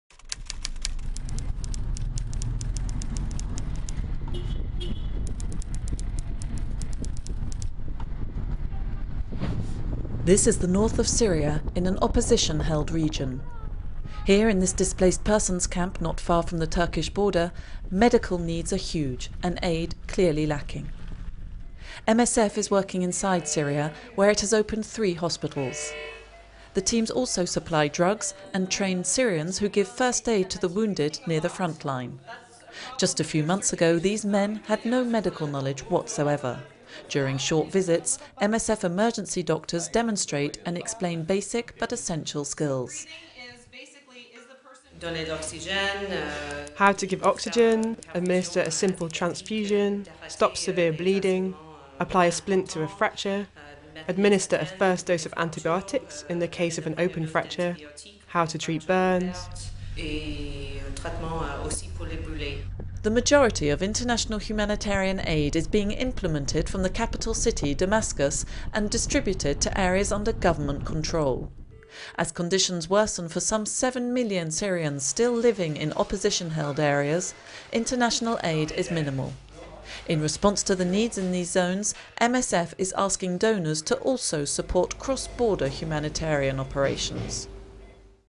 MSF Trains Syrians To Give First Aid at Front Line, Denounces Imbalanced Distribution of Aid
The majority of international humanitarian aid is being implemented from the capital city of Damascus and distributed to areas under government control. Aid remains minimal for some seven million Syrians living in opposition-held regions, where conditions continue to worsen. During a donor conference held in late January, Doctors Without Borders/Médecins Sans Frontières (MSF) spoke out about the imbalanced distribution of international aid provided to Syria. MSF teams are providing care for patients in neglected areas and training Syrians to give first aid near the front line.